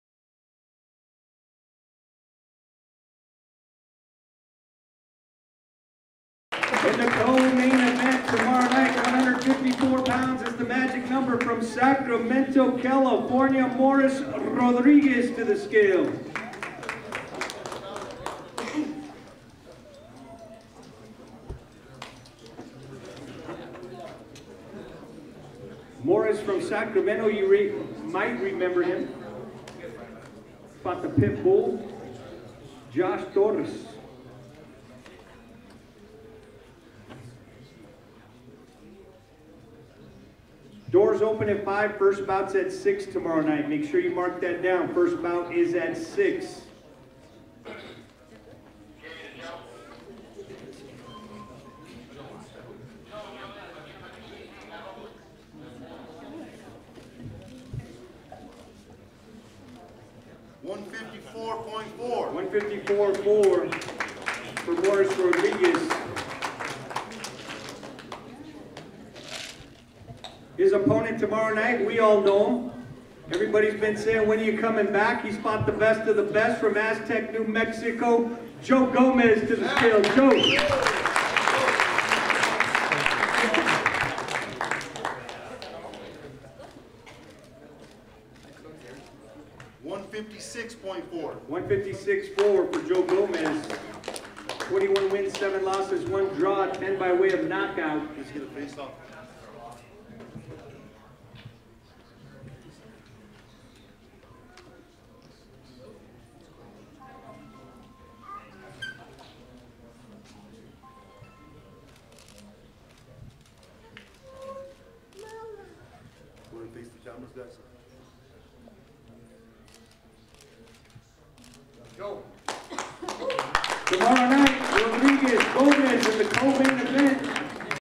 Weigh In Gomez vs Rodriguez
Joe Gomez and Moris Rodriguez hit the scale before their fight Saturday night in Expo Explosion II at Expo New Mexico.
A Legacy Boxing Promotion.  Watch the fights on Proview Sports, Saturday night at 6pm.